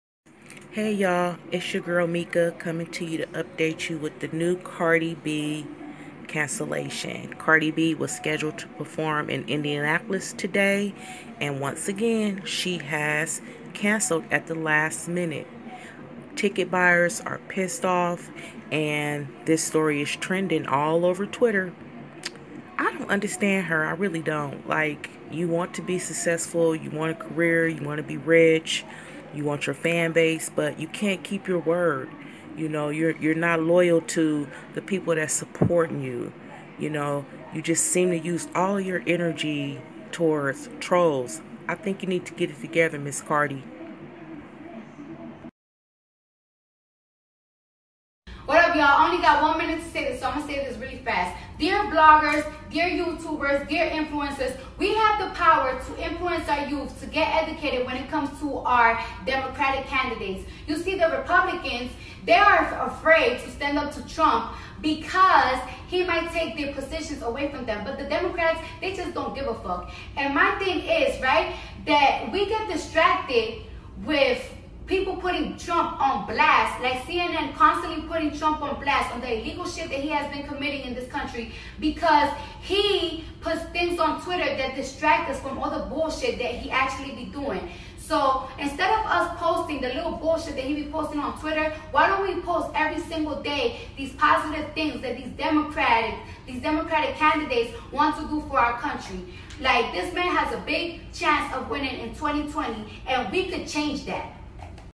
CARDI B CANCELLED SHOW IN INDIANAPOLIS
CARDI B

Bankers Life Fieldhouse announced that Cardi B's sold-out show has been rescheduled for Sept. 11.

In a statement issued at 6:50 p.m., the arena cited "circumstances beyond anyone’s control" for the postponement. Tickets for the July 30 date will be honored in September.

According to multiple police officers and security, the Grammy-winning rapper had been in Bankers Life Fieldhouse for soundcheck prior to the postponement.

    ❗️URGENT❗️

    Tonight’s Cardi B show will not be played as scheduled.

    Rescheduled date is September 11. All tickets for the original date will be honored. Additional details will follow.
    — Bankers Life Fieldhouse (@TheFieldhouse) July 30, 2019